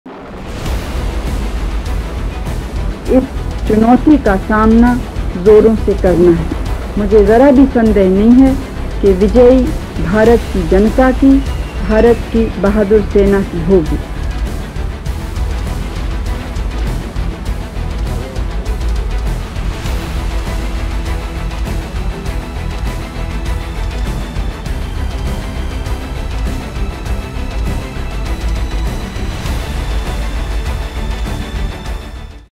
Military Literature Festival Chandigarh 2021 - Promo 2
Experience the most sensational stories of the 1971 Indo Pak war and Discussions on Contemporary Topics.

Date: 11th  - 12th & 18th - 19th, December 2021.

Time - 6 PM onwards.